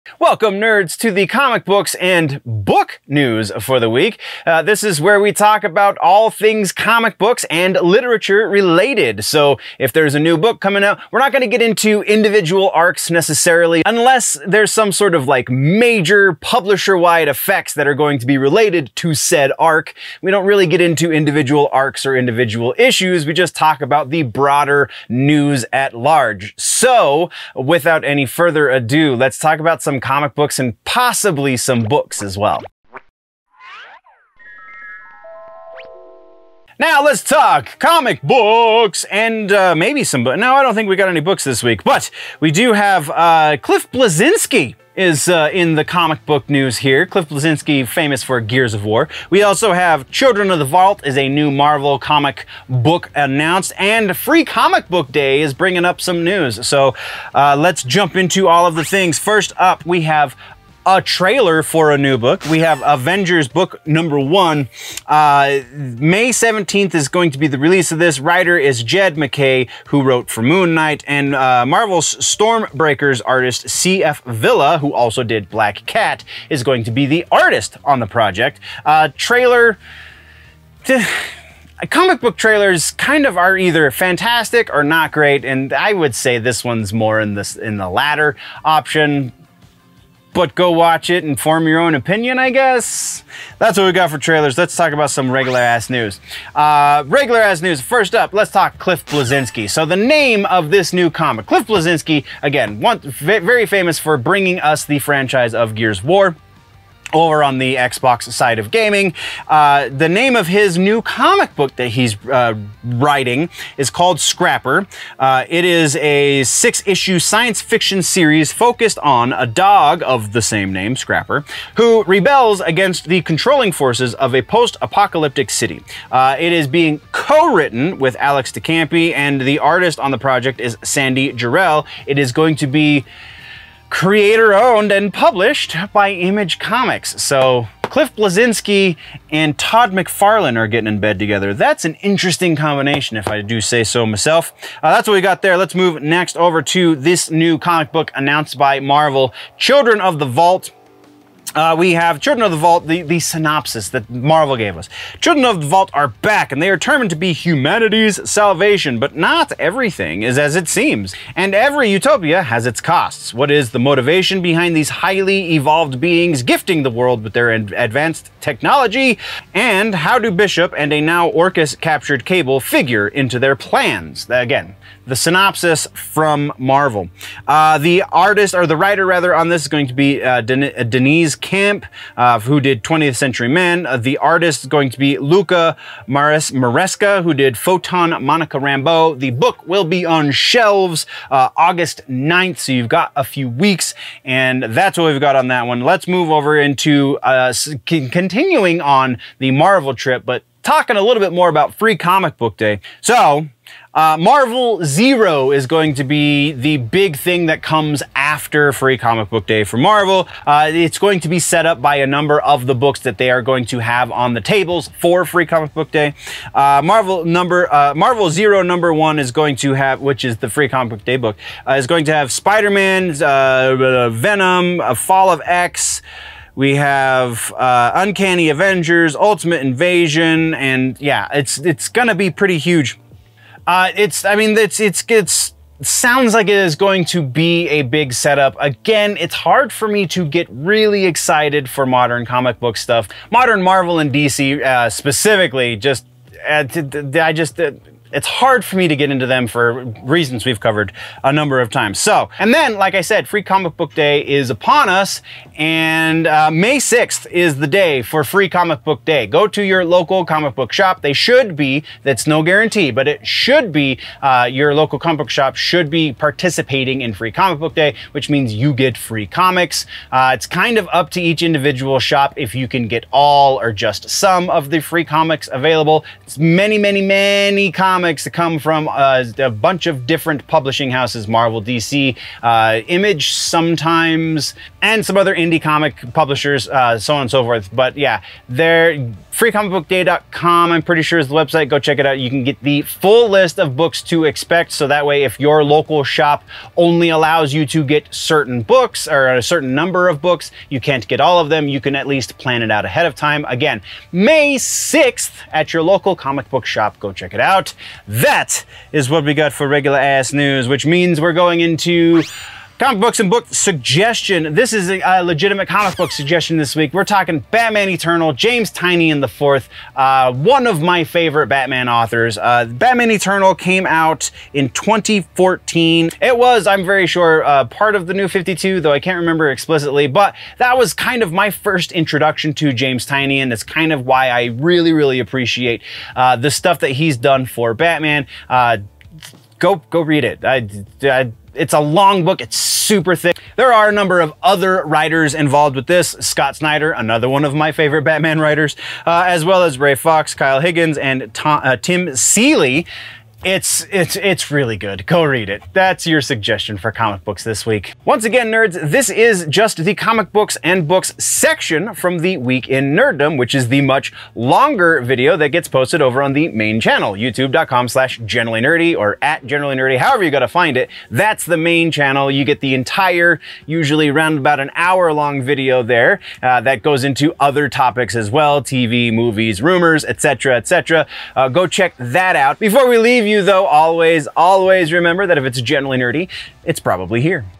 Avengers of the Vault in Marvel Zero | Nerd News
Comic Books/Books news from LAST WEEK

Trailers
Avengers 01 May 17th writer Jed MacKay (Moon Knight) and Marvel's Stormbreakers artist C.F. Villa (Black Cat)

Reg ‘ol News
Scrapper - Cliff Blezinski, known for the Gears of War franchise, is starting his own comic book. It’s a six-issue sci-fi series focused on a dog of the same name who rebels against the controlling forces of a post-apocalyptic city. co-writer Alex de Campi and artist Sandy Jarrell to be published by Image Comics.
Children of the Vault - Marvel announced new series. (W) Deniz Camp (20th Century Men) (a) Luca Maresca (Photon: Monica Rambeau) Aug 9th:
“The Children of the Vault are back, and they are determined to be humanity's salvation! But not everything is as it seems, and every utopia has its costs. What is the motivation behind these highly evolved beings gifting the world with their advanced technology? And how do Bishop and a now-Orchis-captured Cable figure into their plans?”
Marvel Zero - Free Comic Book Day is almost upon us (May 6th) and Marvel is bringing in new jump on points. In Marvel Zero #1:
Spidey faces a new villain, while two classic villains return, setting the stage for the explosive upcoming story arcs in AMAZING SPIDER-MAN!
If you thought you knew how Venom fit into the history of the Marvel Universe, prepare to be proved WRONG with a shocking story that sets up the next huge epic coming to VENOM!
FALL OF X begins! A new threat to mutantkind emerges while the X-Men's HELLFIRE GALA is in full swing-one with a very familiar visage!
And with the world in disarray, the moment calls for a new alliance between heroes both mutant and otherwise-the all-new UNCANNY AVENGERS!
Previews of Jonathan Hickman's two new Marvel projects, ULTIMATE INVASION with Bryan Hitch and G.O.D.S. with Valerio Schiti!

Suggests
Batman Eternal is a year-long weekly limited series published by DC Comics, that began in April 2014. The series featured Batman, his allies, and Gotham City, and was written by Scott Snyder, James Tynion IV, Ray Fawkes, Kyle Higgins, and Tim Seeley. John Layman was originally scheduled to write for the series as well, before leaving the project in January 2014 and being replaced by Higgins; his final issue was #10. Batman Eternal ran through April 2015, after which it took a hiatus, before returning in October 2015 for a 26-issue weekly sequel series titled Batman and Robin Eternal.